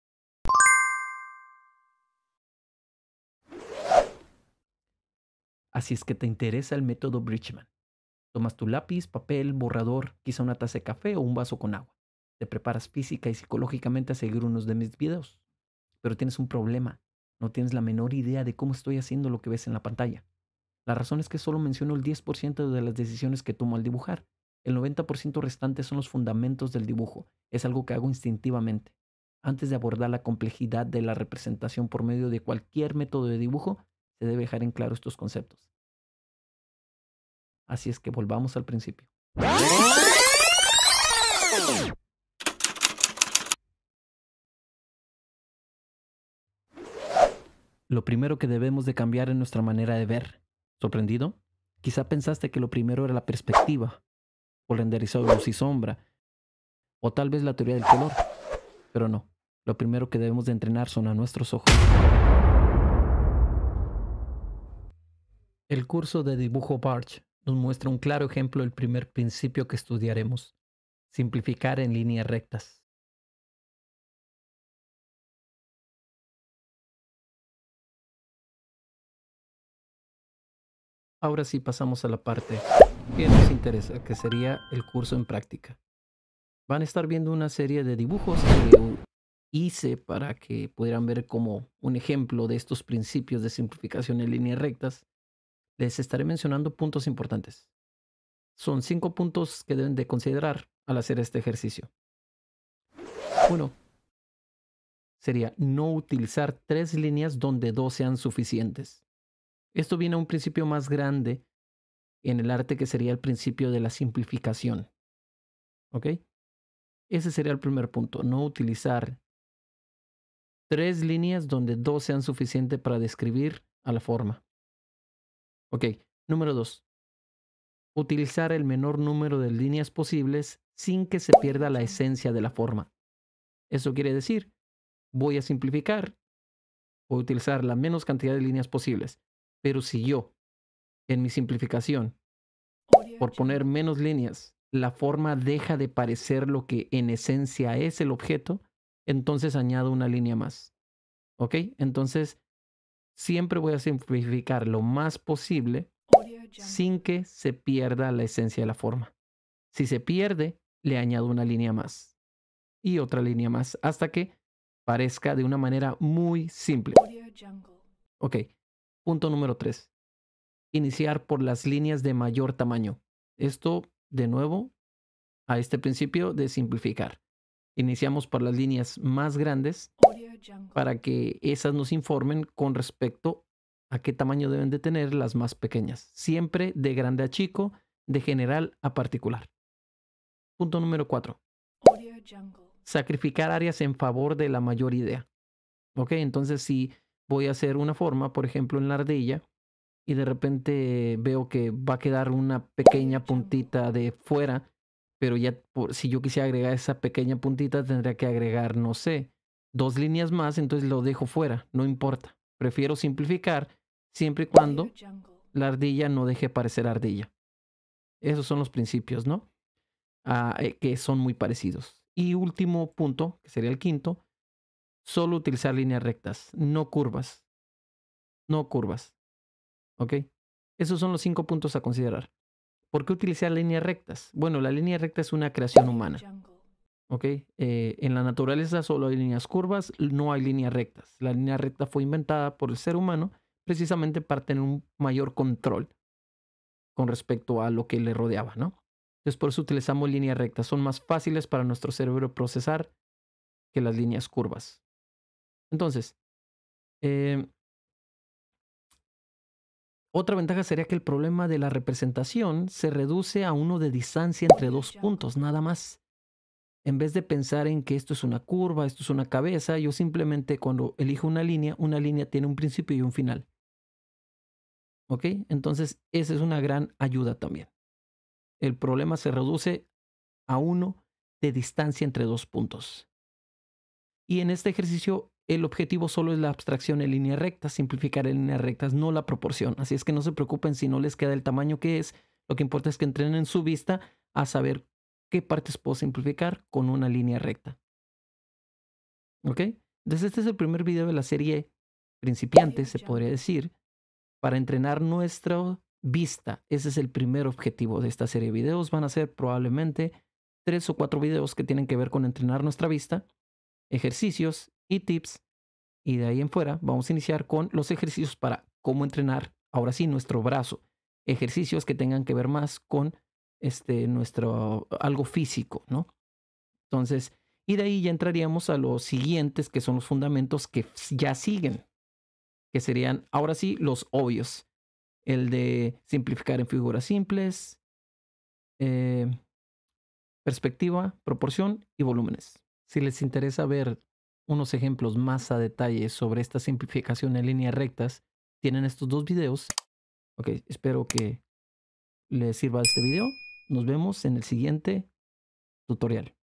[PASO A PASO] como DIBUJAR, PRINCIPIANTE, fundamentos del dibujo parte 1
En este video te mostrare como dibujar fácil, paso a paso cualquier tema, personas, perros, paisajes, insectos,  sin importar estilo puede ser anime, comic, dibujo de la figura humana realista, dibujo académico.
Lo importante es entrenar nuestros ojos a simplificar en líneas rectas.

Este es el primer video de la serie de principiantes en el dibujo, espera los próximos videos tutoriales.